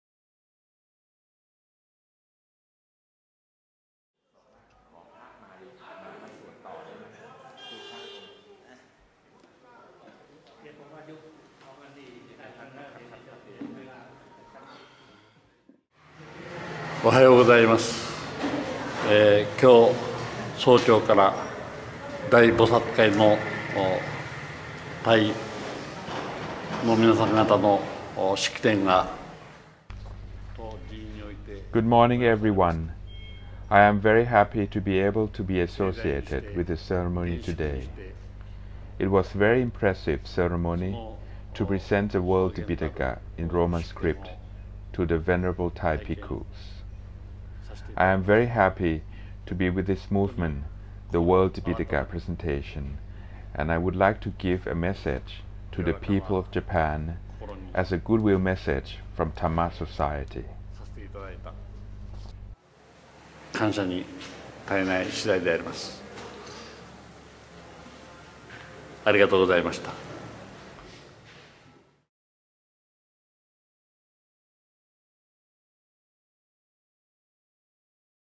World Tipitaka & Buddhagaya (Eng)
In 2007, Her Royal Highness Princess Galyani Vadhana of Thailand, Honorary President of Tipiṭaka Presentation Worldwide, graciously presented as a gift of Dhamma, &quot;The World Tipiṭaka Edition in Roman script (40 volumes)&quot; to Maha Bodhi Society of India, the first institute in the Republic of India to have received the royal gift.

To follow the Princess's magnanimous initiatives, Dhamma Society in 2008 printed  additional 600 sets of this year's Chanting Edition (Majjhimanigāya in 3 volumes), as requested by Maha Bodhi Society. These Chanting Editions will be presented to participating Venerable Buddhist monks from all over the world.

The 3rd Annual Pāḷi Tipiṭaka Chanting is  organised between February 12-22, 2008 by Maha Bodhi Society of India at Buddhagaya, the holy site where the Buddha attained enlightenment almost 3,000 years ago.

At the opening ceremony, Maha Bodhi Society has dedicated the merits of this year's activity to the Late Princess Patron of the World Tipiṭaka.

In this clip, Mr Hideo Okada, standing in front of the Royal Gift of the World Tipiṭaka, sendind a good-will message of Dhamma Society from the Maha Bodhi Society's Vihara at Buddhagaya, India.

Digital Archives from Dhamma Society's World Tipiṭaka Project in Roman Script, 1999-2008.